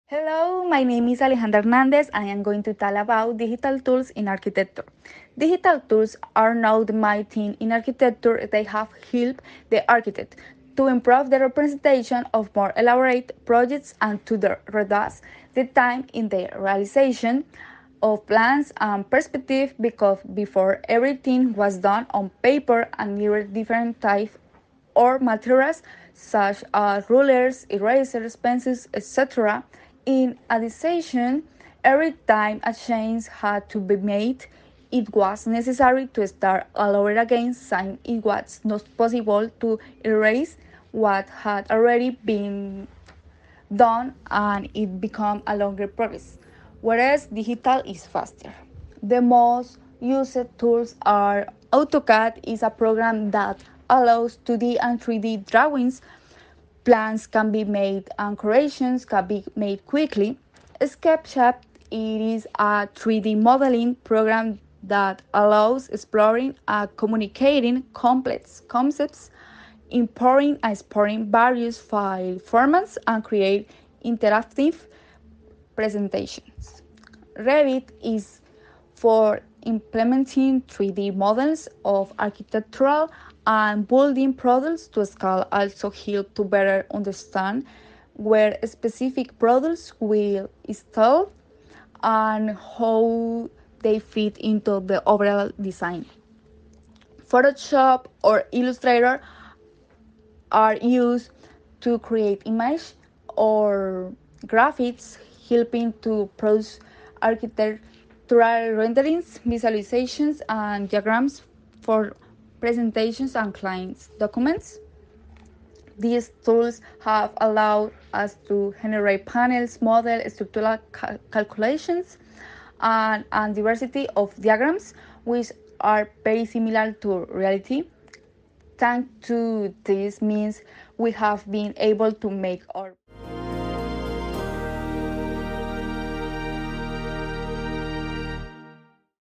VIDEO EXPRESSION V ALEJANDRA HERNANDEZ
Digital tools are now the main thing in architecture, they have helped the architect to improve the representation of more elaborate projects and to reduce the time in the realization of plans and perspectives because before everything was done on paper and needed different types of materials such as rulers, erasers, pencils, etc..
In addition, every time a change had to be made, it was necessary to start all over again since it was not possible to erase what had already been done and it became a longer period of time, whereas digital is faster.
the most used tools are:
AutoCAD is a program that allows 2D and 3D drawings, plans can be made and corrections can be made quickly.
Sketchup: It is a 3D modeling program that allows exploring and communicating complex concepts, importing and exporting various file formats and creating interactive presentations.
Revit : Is for implementing 3D models of architectural and building products to scale also helps to better understand where specific products will be installed and how they fit into the overall design.
Photoshop: is used to create images and graphics, helping to produce architectural renderings, visualizations and diagrams for presentations and client documents.
These tools have allowed us to generate panels, models, structural calculations and a diversity of diagrams which are very similar to reality, thanks to these means we have been able to make our projects known to more people and places.